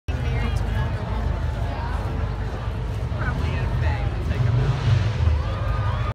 2017 Raleigh Christmas Parade Watch Party at Dram & Draught - YMCA Super Skippers
We had a blast hosting everyone at Dram & Draught for our inaugural Raleigh Christmas Parade Watch Party on Saturday, November 18, 2017. Even Santa showed up! We'll definitely be doing this again next year and promise to order more Bailey's, coffee, hot chocolate, and cider - you all sure like your breakfast cocktails... cheers!